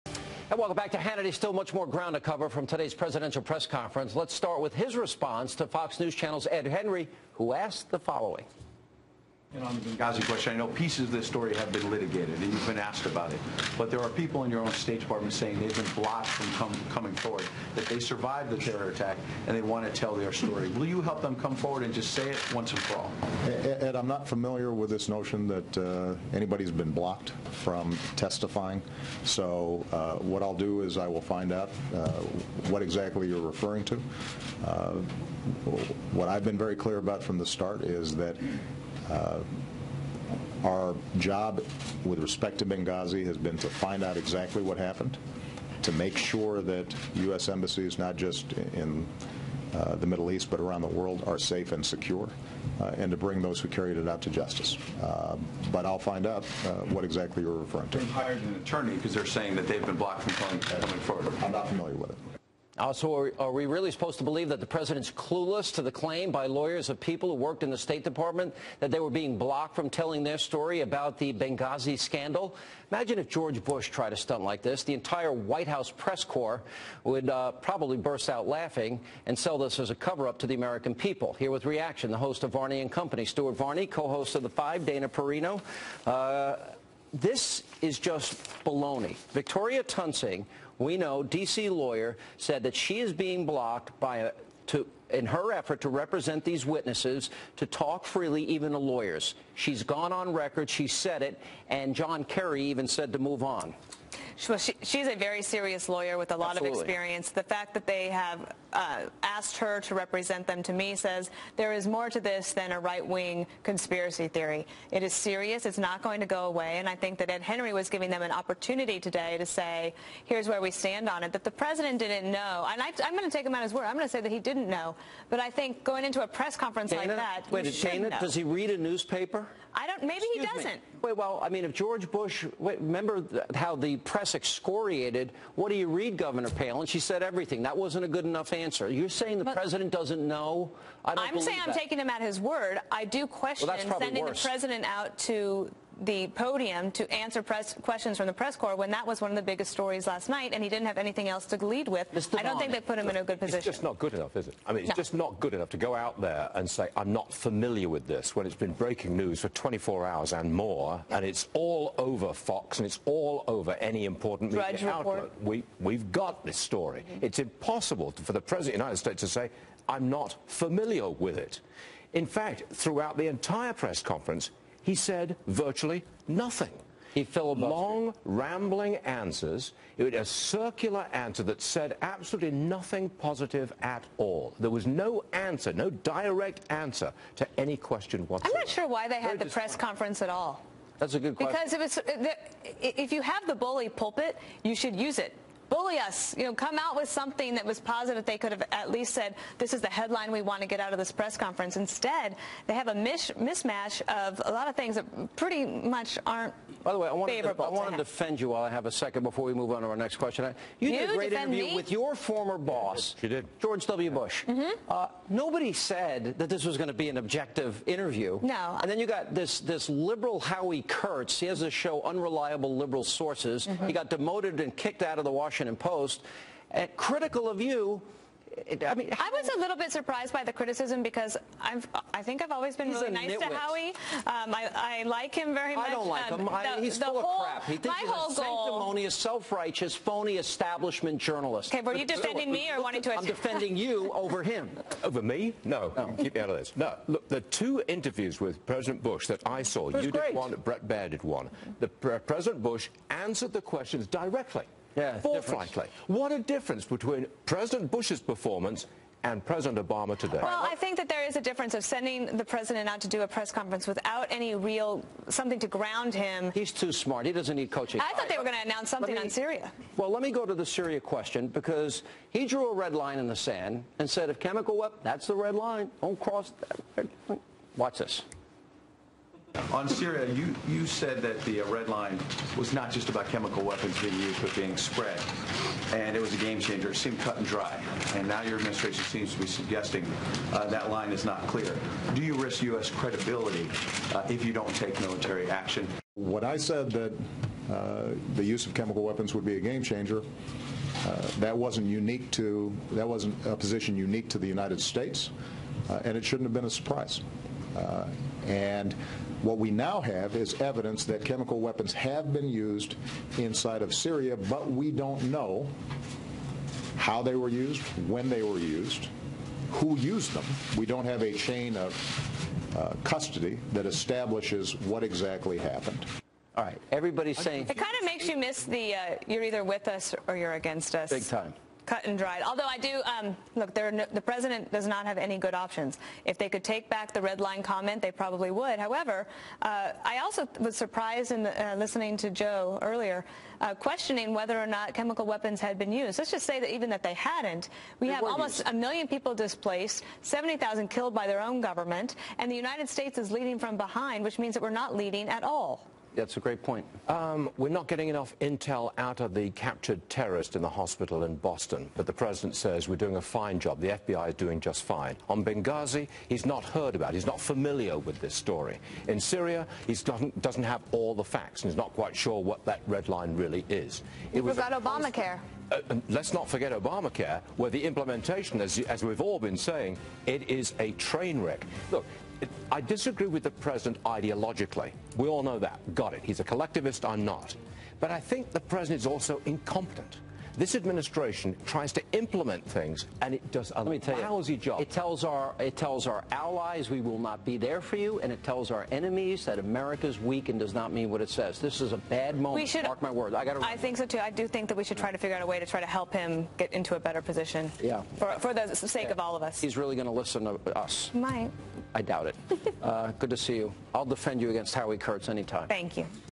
Does Obama know ANYTHING about ANYTHING?
Dumbo's press conference seems to be nothing more than the ramblings of a madman who is on the verge of  losing it. After seeing "the most intelligent man alive" trying to answer some basic questions I do believe Obama has earned himself the Stupid award.

"The state must declare the child to be the most precious treasure of the people. As long as the government is perceived as working for the benefit of the children, the people will happily endure almost any curtailment of liberty and almost any deprivation." -Mein Kampf, Adolf Hitler